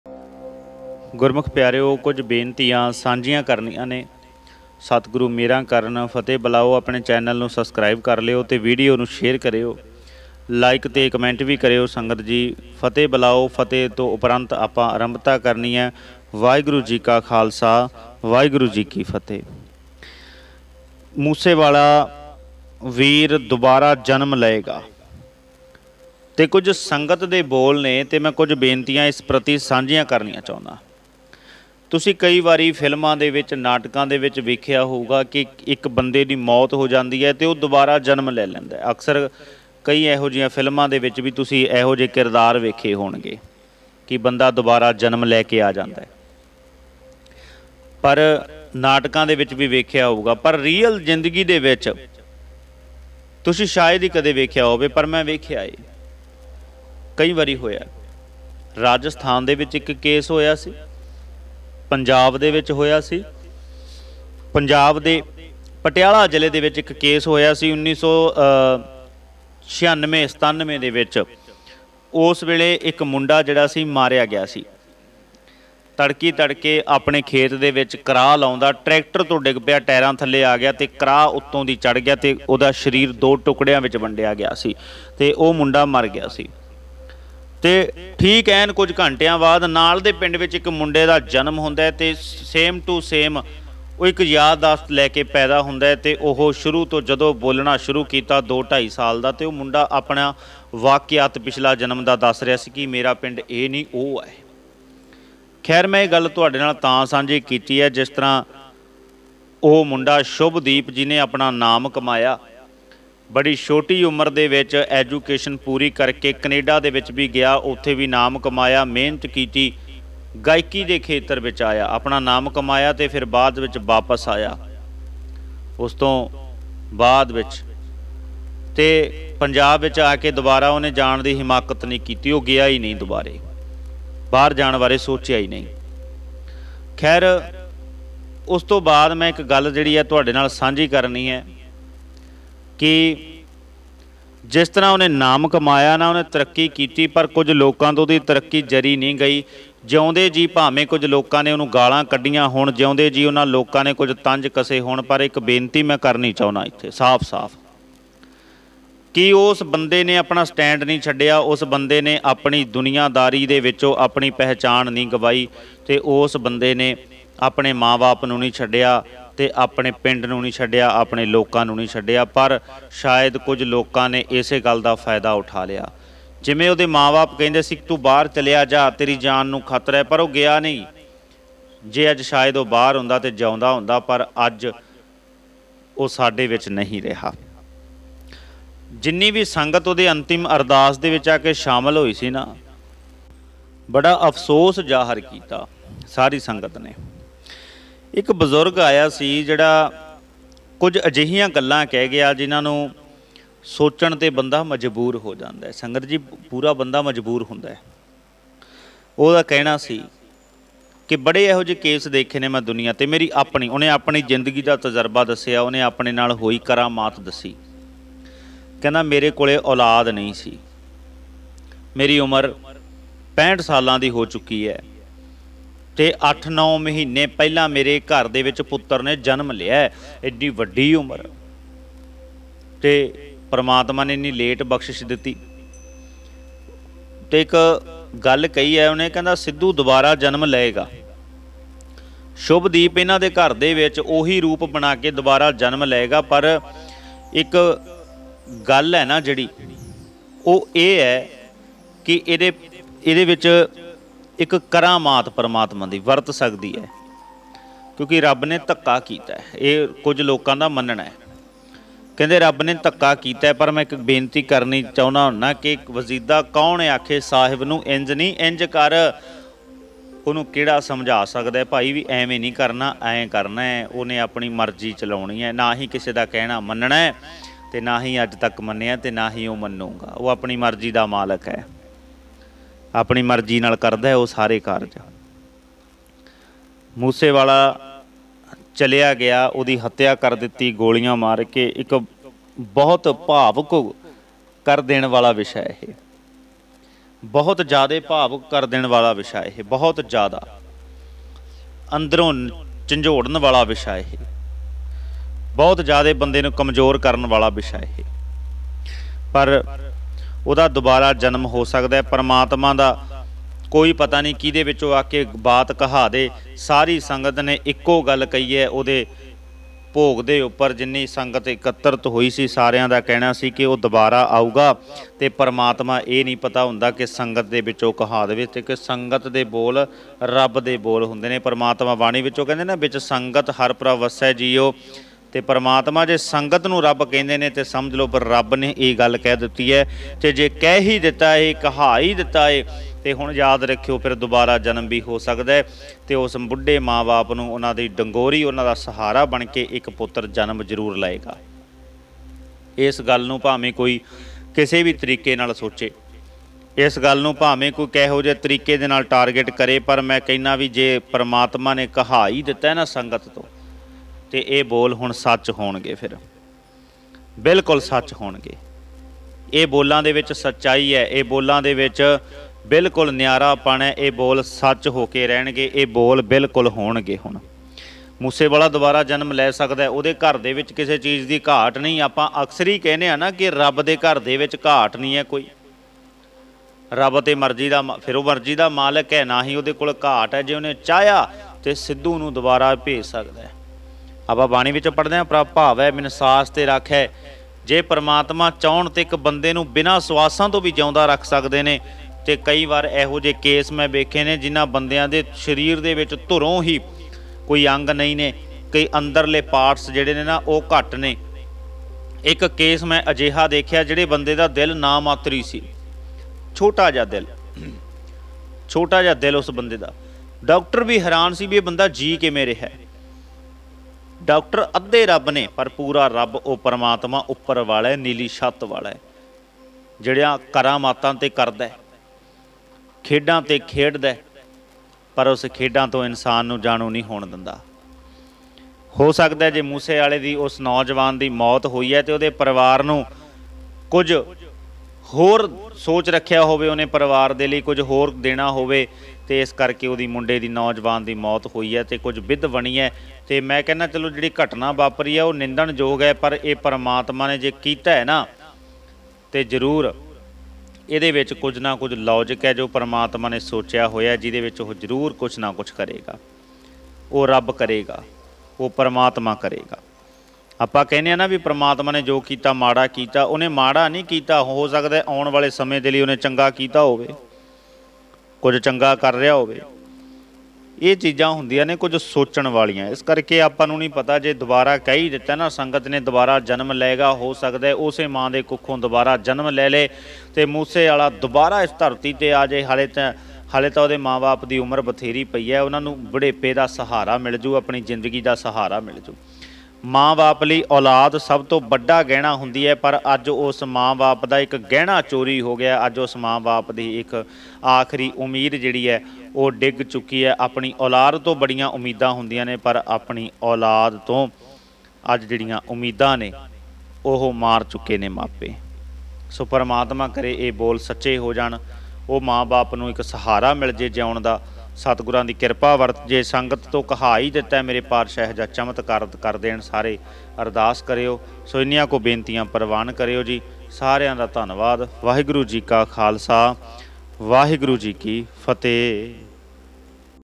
ਇਸ ਸਾਲ ਇਹ ਤਾਰੀਕ ਮੂਸੇਵਾਲਾ ਬਾਈ ਦੁਬਾਰਾ ਜਨਮ ਲਵੇਗਾ ਇਸ ਬਾਬੇ ਨੇ ਕੀਤੀ ਭਵਿੱਖਬਾਣੀ
Voice - Bhai Inderjeet Singh Ji
Your Comments are very Important to us. DhanGuruGobindSinghJi